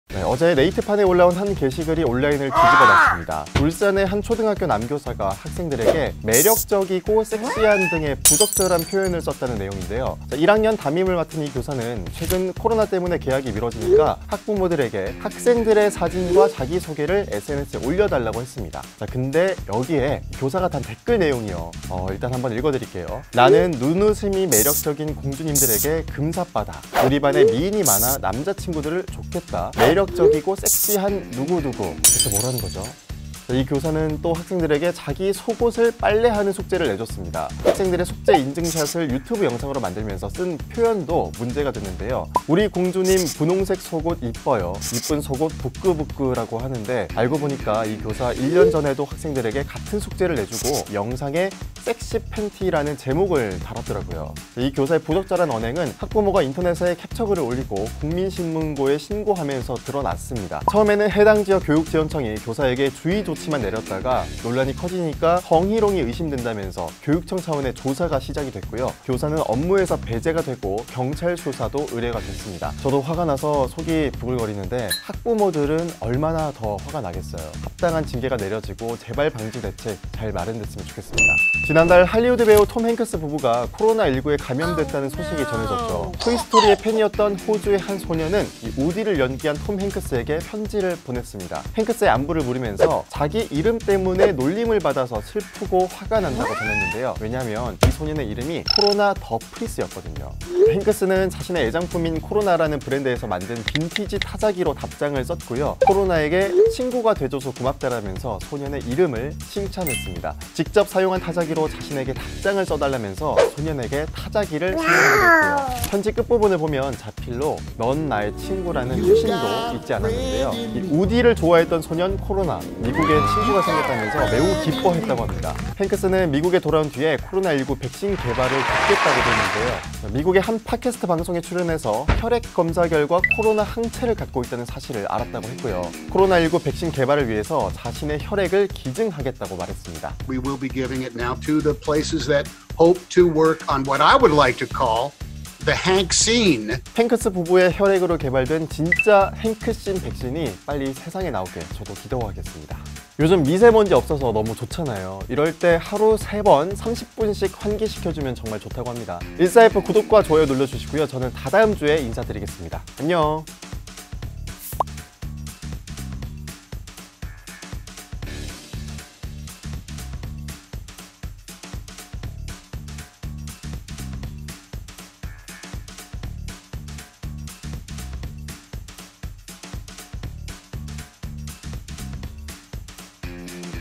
팬티 빨기 숙제 내주고 ‘섹시팬티’ 영상 만들어 올린 초등학교 1학년 담임교사 / 14F
울산의 한 초등학교 1학년 담임교사의 부적절한 언행이 논란이 되고 있습니다. SNS 단체대화방의 학생 사진에 ‘매력적이고 섹시한’이라는 표현을 사용하는가 하면, 학부모로부터 민원을 받은 이후에도 속옷 세탁을 숙제로 내준 뒤 ‘이쁜 속옷, 부끄부끄’와 같은 댓글을 달아 문제가 됐는데요. 울산교육청은 이 교사를 업무에서 배제시키고 성희롱이 의심된다며 경찰에 수사를 의뢰했습니다.